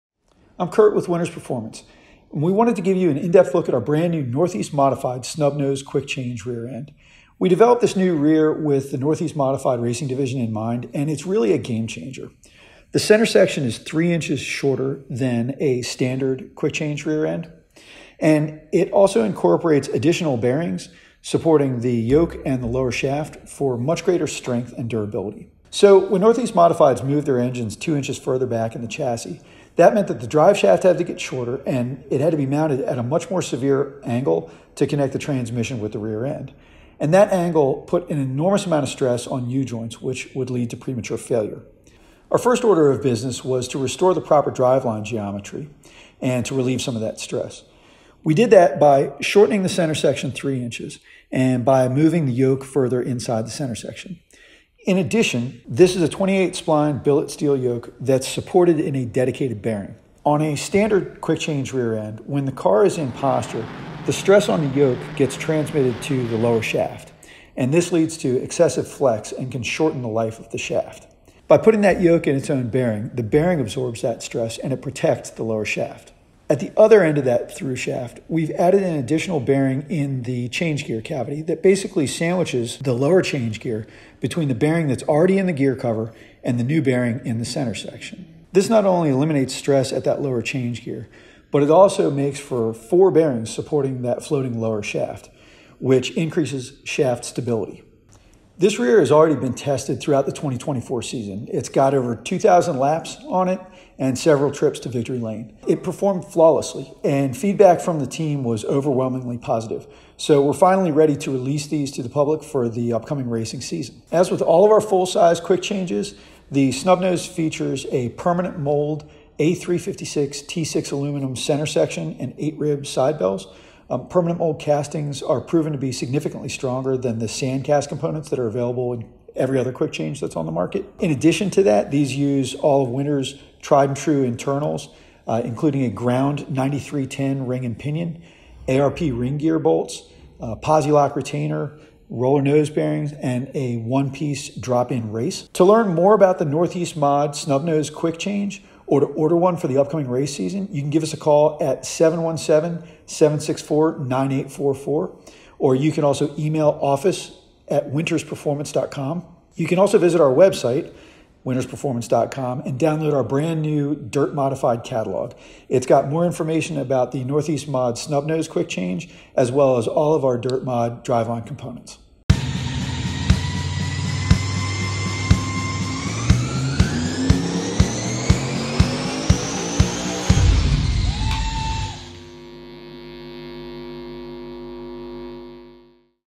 New Northeast Mod Snub Nose Quick Change // Winters Tech
Our new Northeast Mod Snub Nose Quick Change revolutionizes Modified rear end design. 3" shorter center section increases driveshaft length and improves driveline geometry, saving U-joints and increasing reliability. Floating lower shaft and added bearings (including an additional bearing interposing the lower change gear) greatly improve strength and durability. And Permanent Mold castings make these the strongest full-size production quick changes on the market.

Chapters:
0:00 Intro
0:25 Driveline geometry 101
0:41 Geometry improvements
0:53 Yoke support bearing
1:15 Floating lower shaft & change gear support
1:38 Testing the Snub Nose
1:53 The Permanent Mold Advantage
2:10 Ring & pinion, hardware, and internal specs
2:25 New catalog and Snub Nose ordering info

Since 1958, Winters has been crafting the highest quality performance rear ends and driveline components for racers and hot rodders alike.